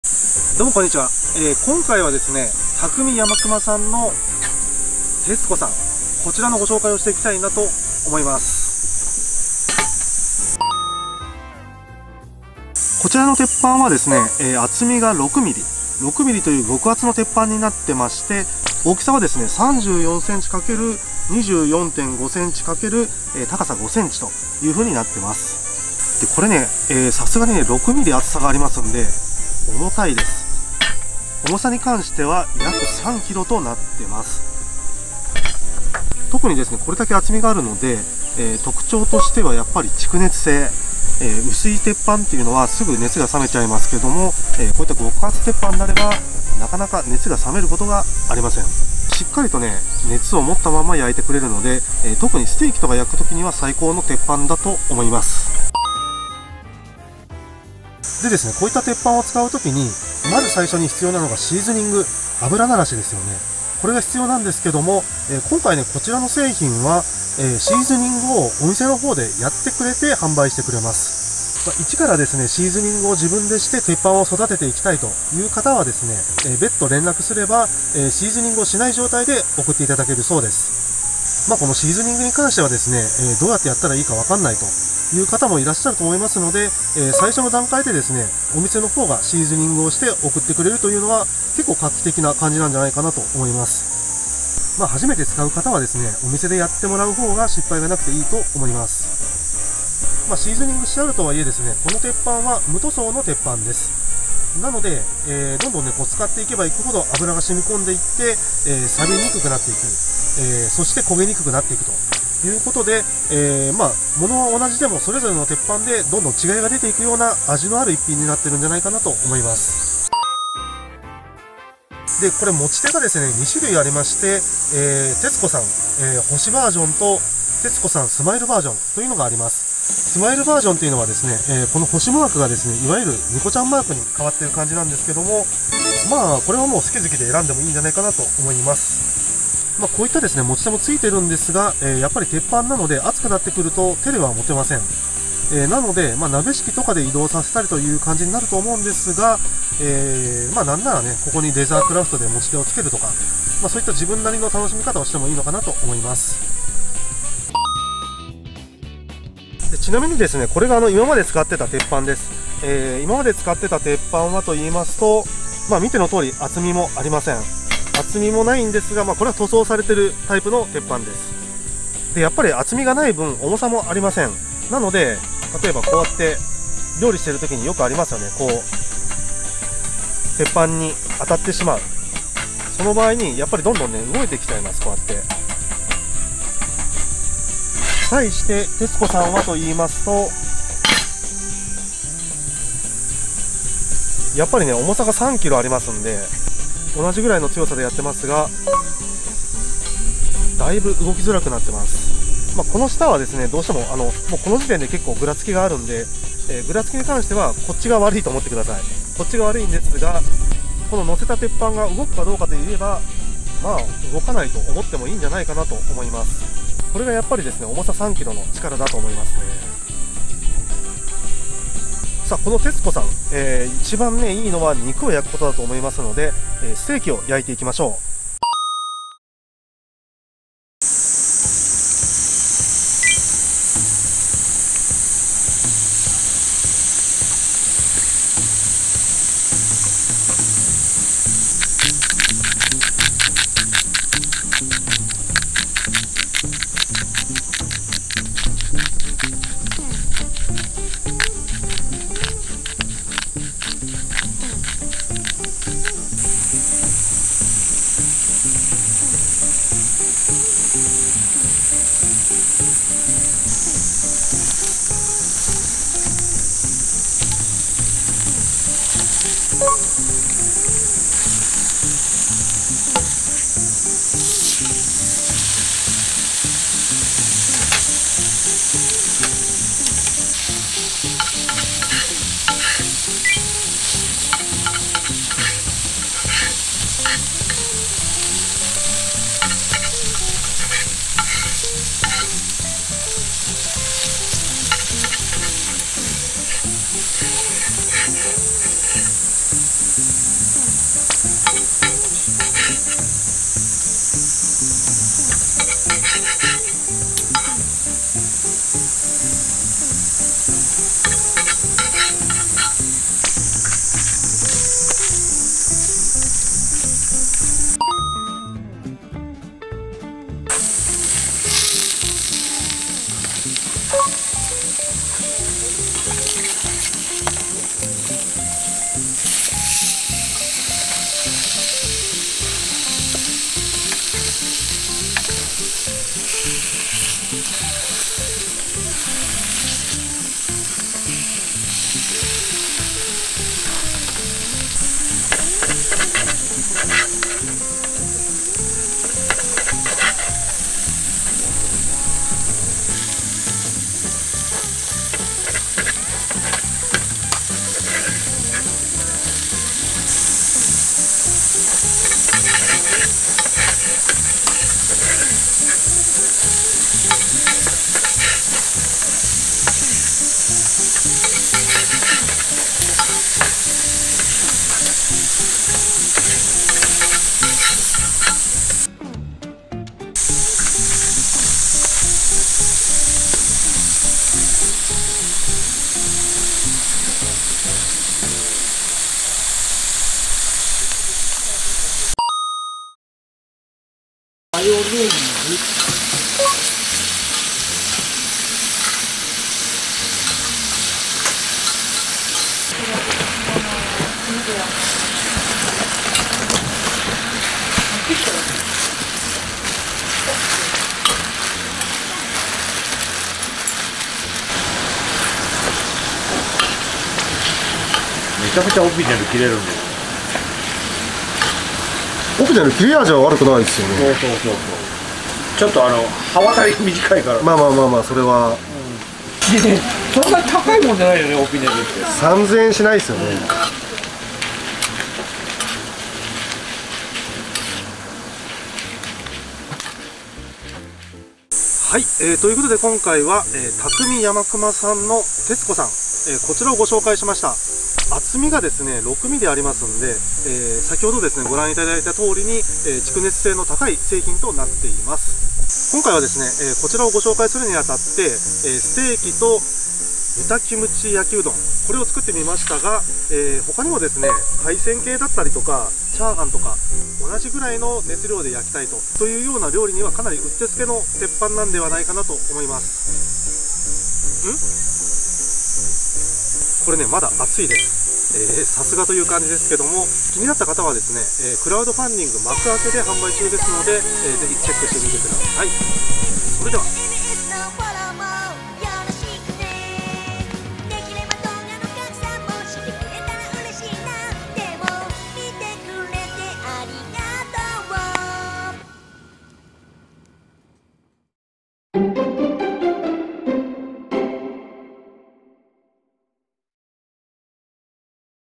今しか買えない！キャンプ＆BBQに最適な極厚鉄板をどうぞ【匠・山熊「鉄子さん」】
今回はマクアケでクラウドファンディング中の匠・山熊さんの「鉄子さん」をご紹介します。
残り日数が少なくなっていますので、購入はお早めにどうぞ！

無塗装の鉄板なのでシーズニングが必要ですが、最初のシーズニングをした状態で購入することができます。
（希望によりシーズニングしない状態でのお届けも可能）

「鉄板を使ってみたいけどシーズニングのやり方がよく分からない…」という方に嬉しいサービスだと思います。

シーズニングされて油ならしされているので、ステーキを焼いてもくっつきにくく、繰り返し使って行く事でより「錆びにくく、くっつきにくい」鉄板に育てるのも楽しみの1つじゃないでしょうか？

※使用後はタワシなどで汚れを洗い落とし、水気を切って空焼きして冷めたら油を塗ればOK

鉄子さんは厚さ6mmの極厚鉄板で、3kgの重さがあるだけに蓄熱性と安定感が凄い…。
食材を乗せても鉄板が冷めずに調理を続けられます。

鉄板を紹介するのにはやっぱり実際に肉を焼くのが一番だろうと思って別の鉄板（こちらは薄い塗装されている物）と焼き比べてみましたが、うまく伝わるかどうか…。

同じような火力でレアに調理しても、

鉄子さん→レアだけど中まで熱が通って本当の意味でレアの焼き加減
別の鉄板→表面にしか熱が通っていなくてレアというより生の焼き加減

といった違いが感じられました。
この辺りはとても厚みが関係していると感じます。

この鉄板は少し高めのフチもつけられているので、食材をこぼすことなく調理がしやすいと思います。
クラウドファンディング残り日数もあとわずか。ご購入はお早めにどうぞ～！

使用しているキャンプ道具
シングルバーナー SOTO ST-330

YOLER折りたたみコンロ

スパッタシート

バトニングナイフ SCHRADE SCHF36

ステンレスオピネル8番

スケータースキレット